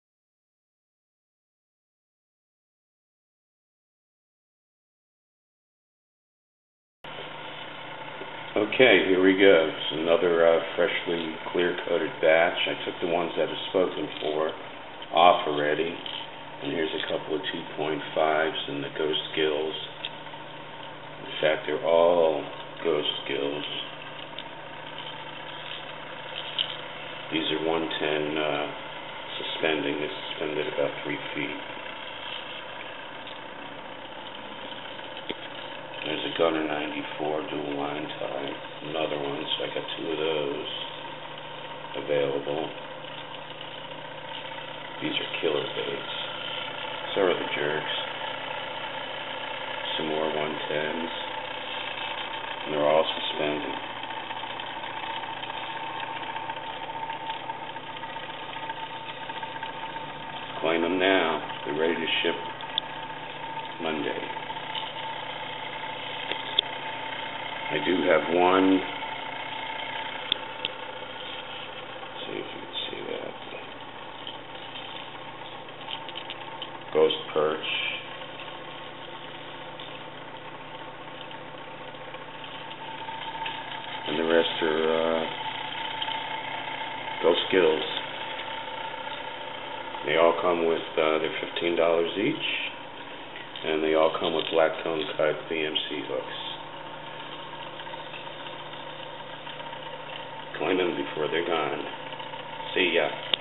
Richard PragerCustom Painted Baits..............Ghost Gill
Ghost Gill in a few different models. All baits shown are available to claim now and ships on Mondays $15 shipped with VMC hooks.